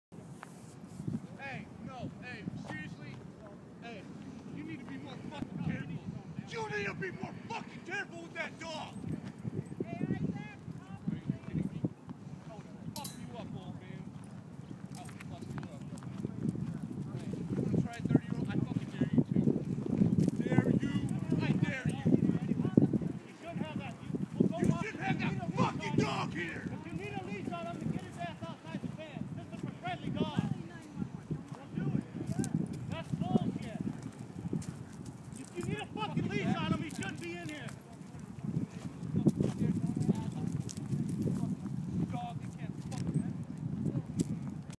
Rose Mofford Sports Complex Dog Park
The regulars at Rose Mofford Dog Park are CRAZY!